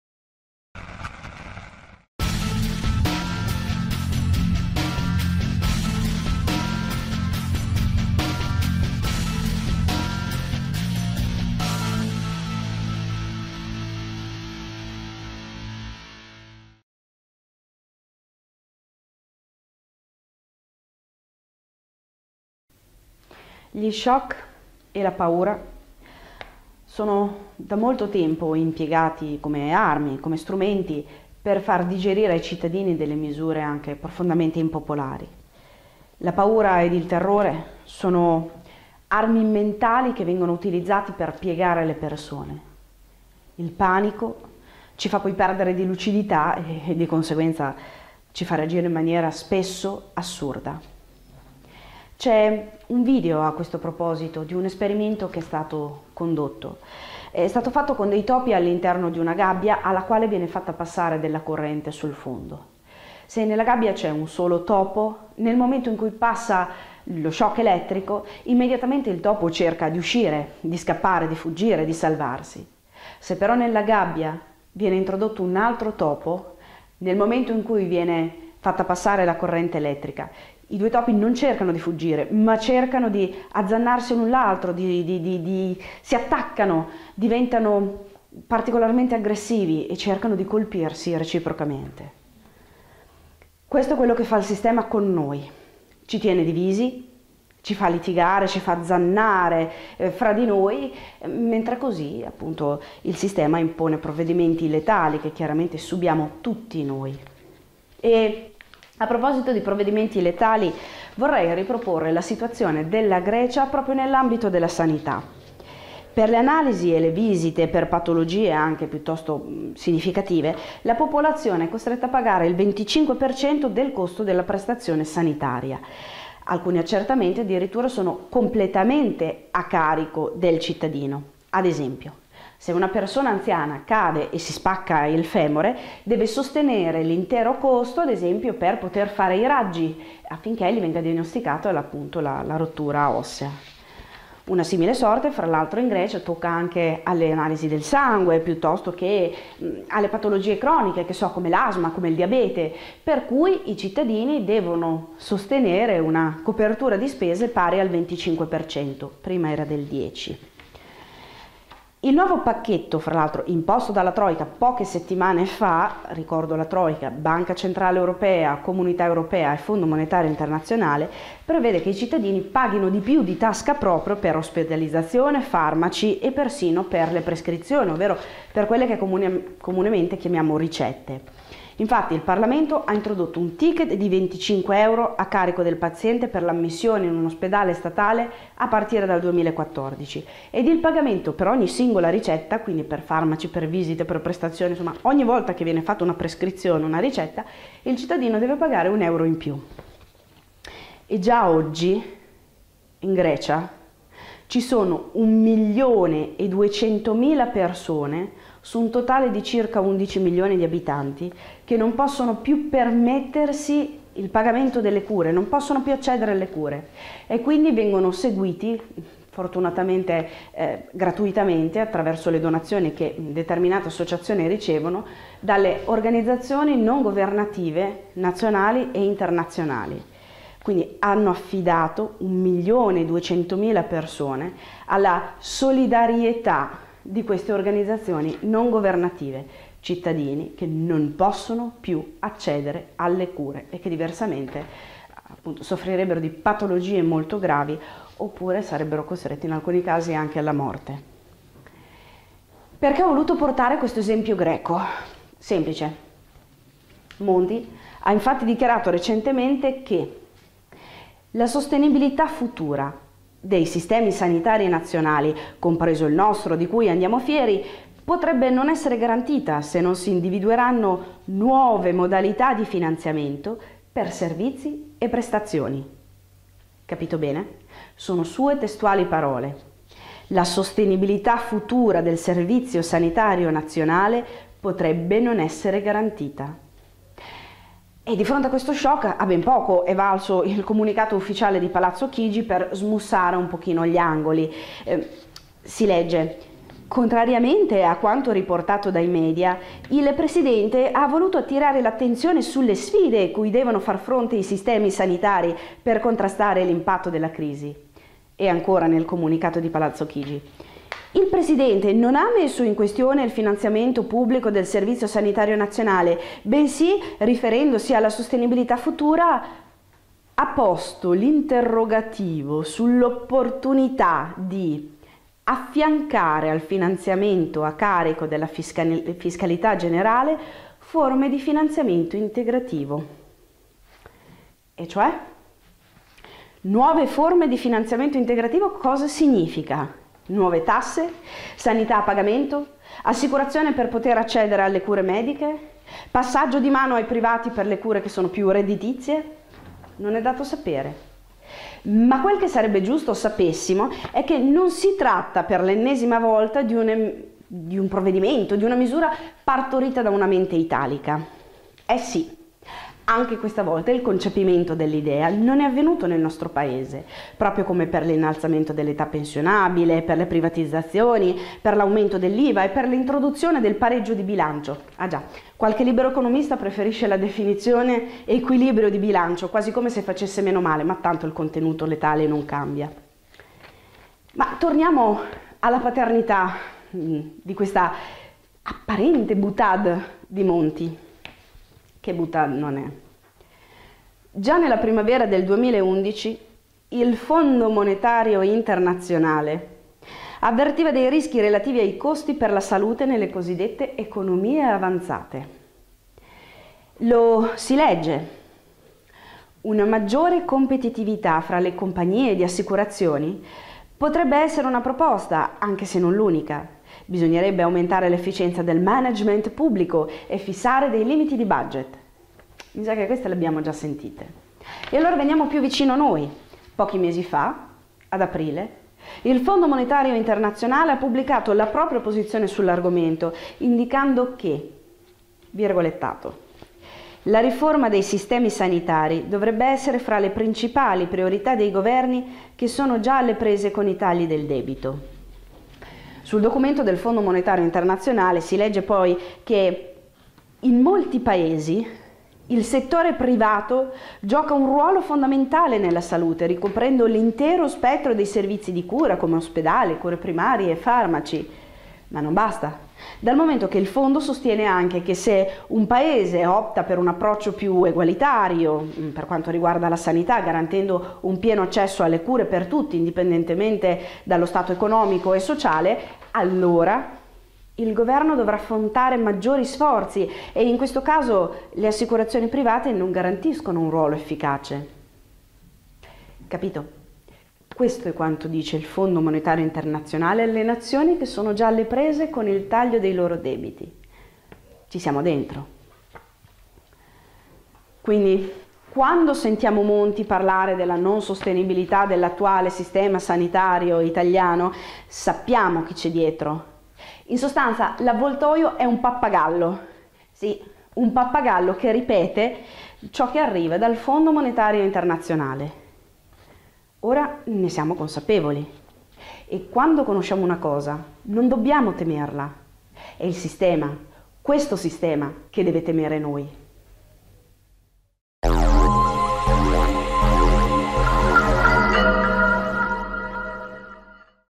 L'avvoltoio è un pappagallo
- Monti dice che il sistema sanitario italiano potrebbe non essere garantito in futuro. In altre parole, non ci sono i soldi. Quindi? Privatizzazioni, assicurazioni, o cessione ai privati dei settori più redditizi? Non si sa. Dice solo che potrebbe non essere garantita, ripetendo così a pappagallo le parole del Fondo Monetario Internazionale. Perché anche in questo caso, come per l'innalzamento dell'età pensionabile, per l'incremento dell'Iva, l'introduzione del pareggio di bilancio, l'FMI non ha perso l'occasione per dire al governo cosa fare anche in materia di sanità